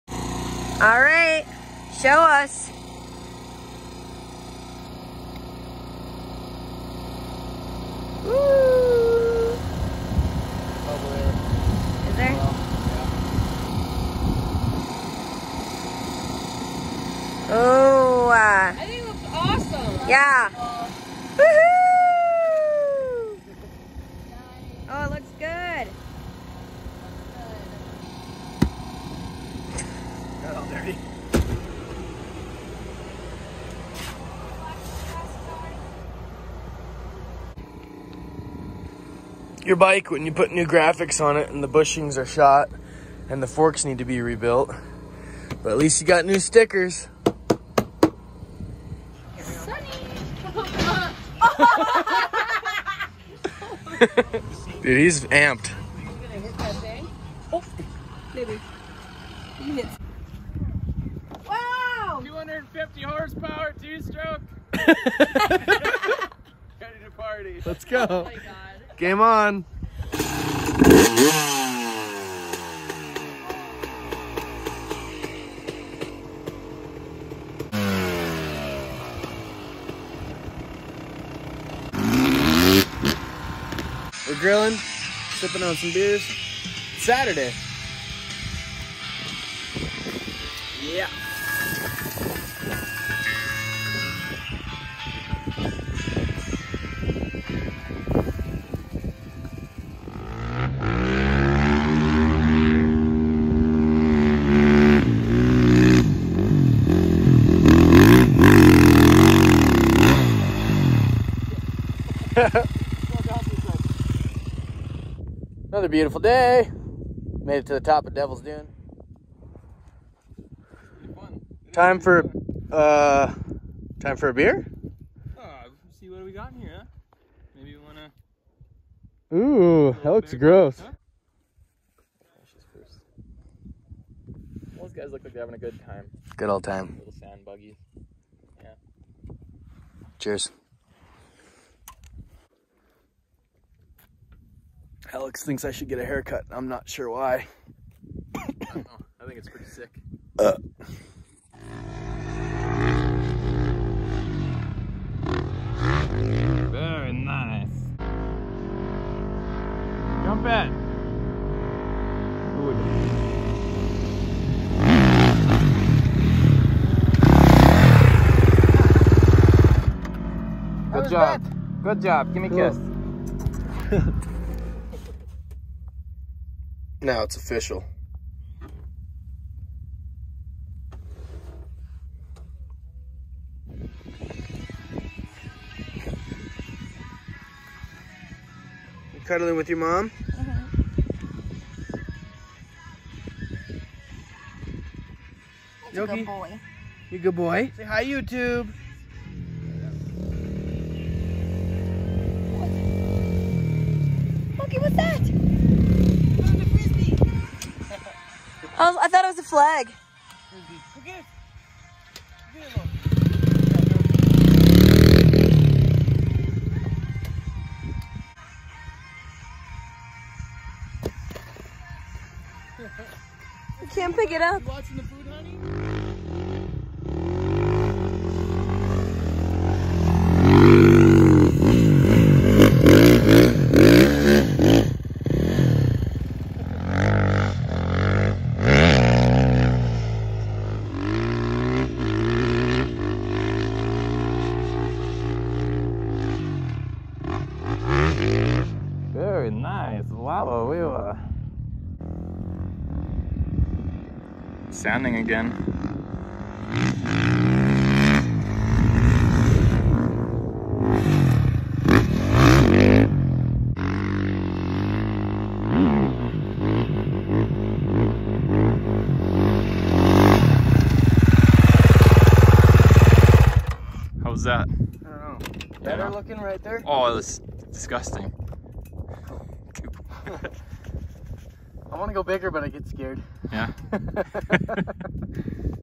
DUNE DAY | Vlog 9 | St. Anthony Idaho Sand Dunes
Bekky is back and she's ready to party. She also picked up a YZ250.